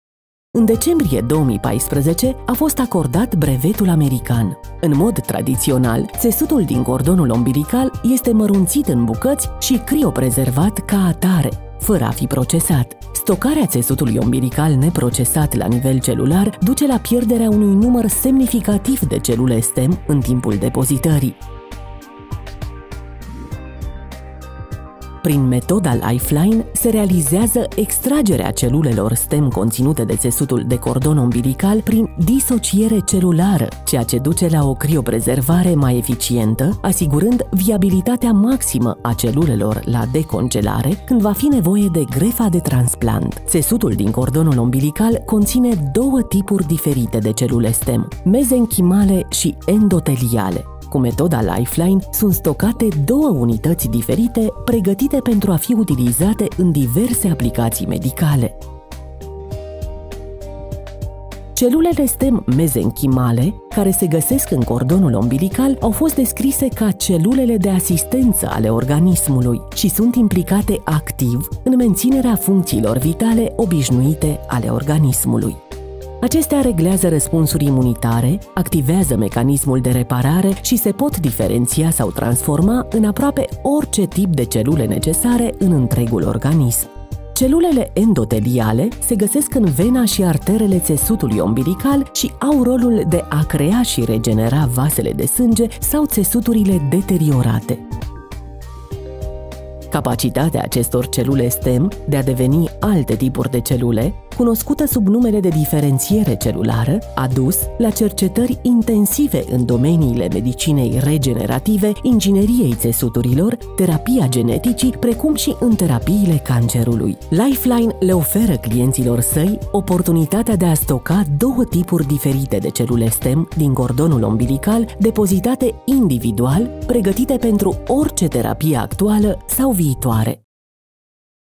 LifeCord - Metodă unică de separare și stocare a 2 tipuri de celule stem din cordonul ombilical
Prin metoda Lifeline se realizează extragerea celulelor stem conținute de țesutul de cordon ombilical prin disociere celulară, ceea ce duce la o crioprezervare mai eficientă, asigurând viabilitatea maximă a celulelor la decongelare, când va fi nevoie de grefa de transplant. Află mai multe